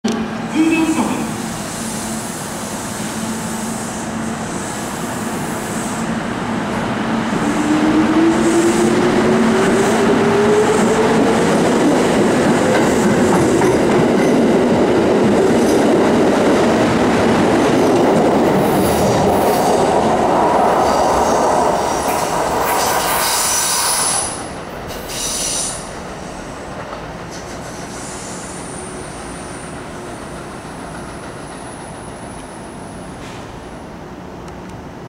近鉄30000系特急鳥羽行き発車
撮影地：宇治山田